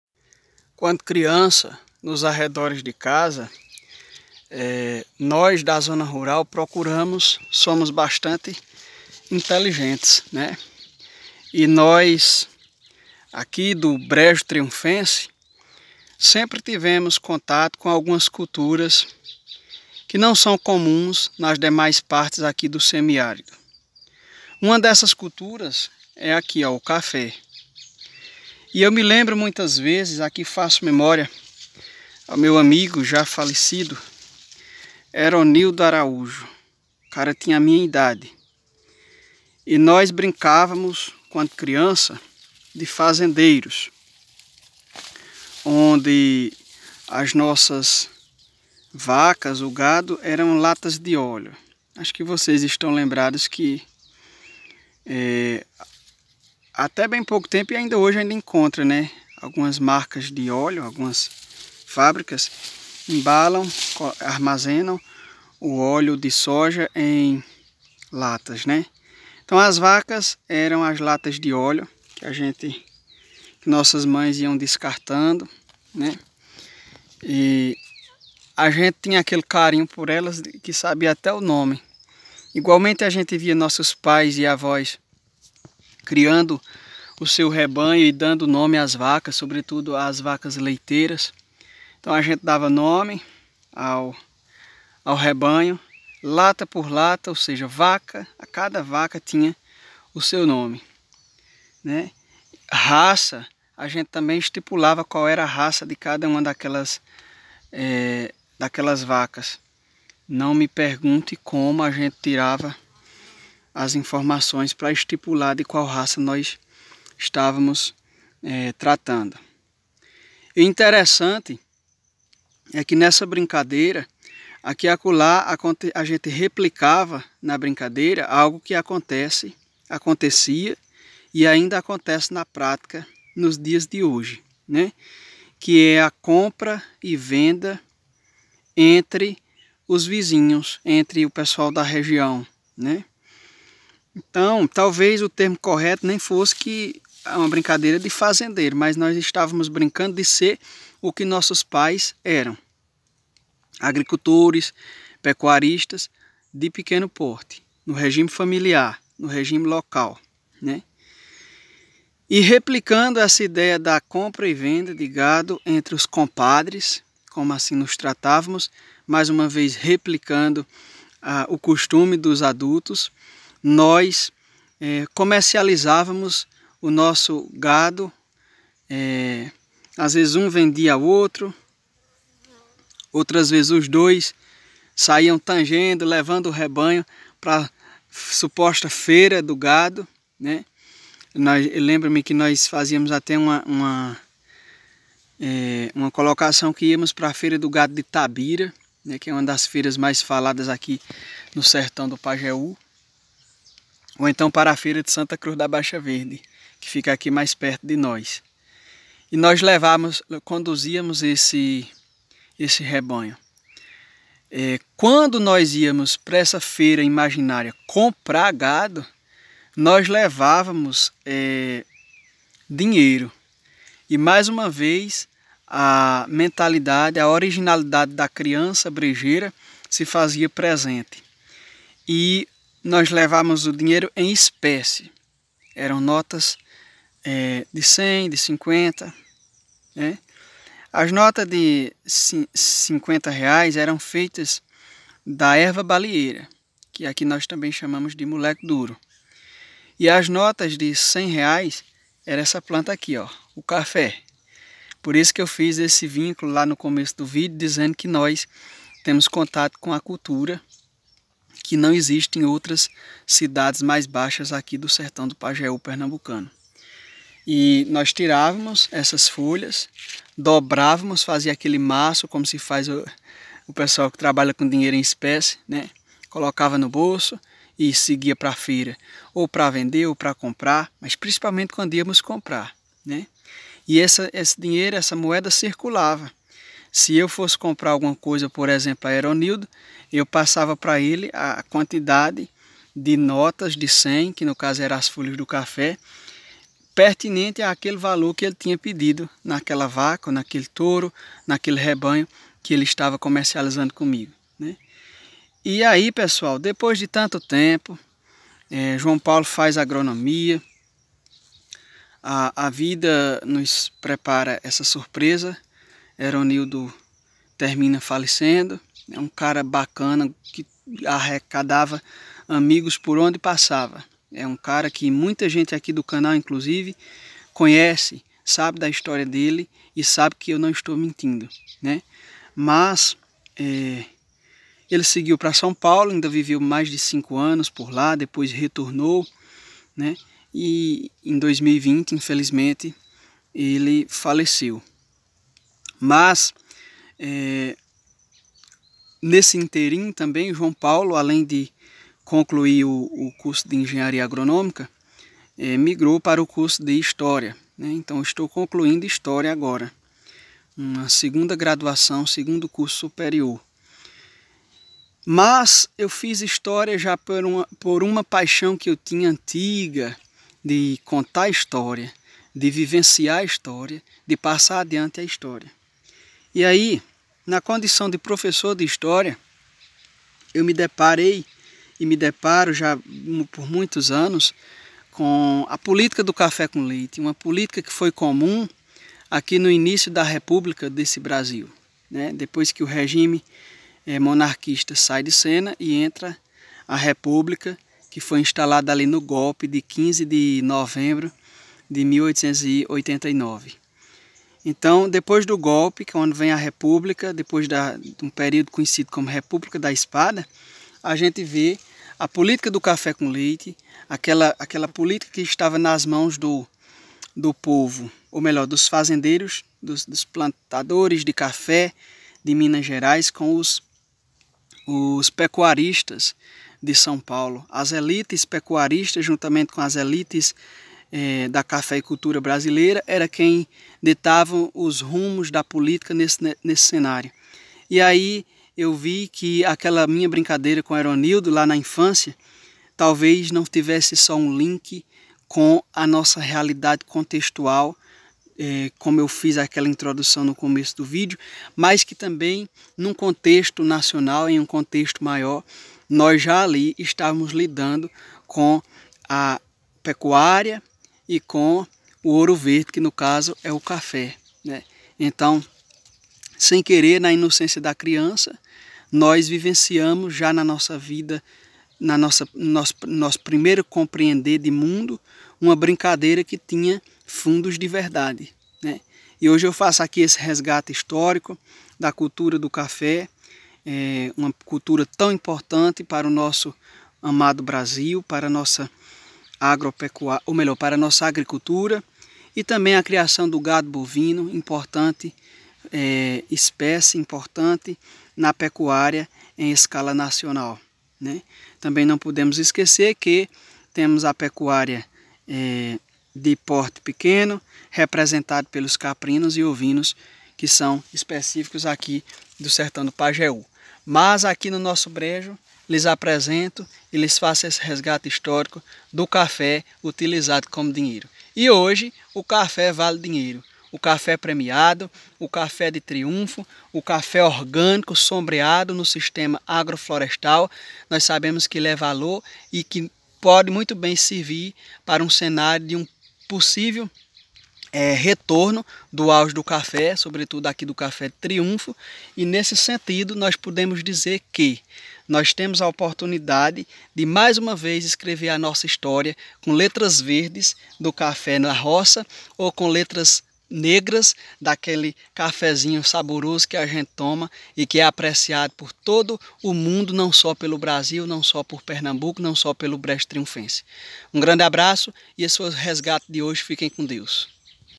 UMA HISTÓRIA COM O CAFÉ
A cafeicultura tem grande vínculo com a história agrícola do País. Pelas análises atuais, ela permanecerá no auge por muito tempo!